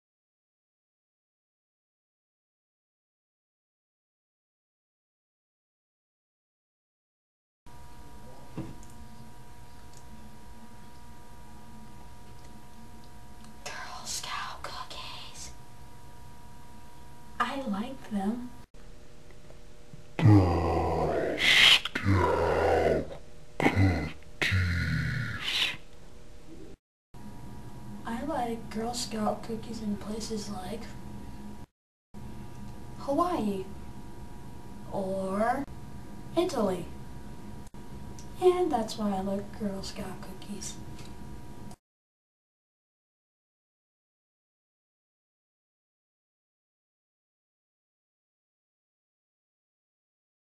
Girl Scout COOKIES!!
I've had them and I really like them. So everyone should also like Girl Scout Cookies. And I like Boy Scout popcorn and pizza also! And even comment.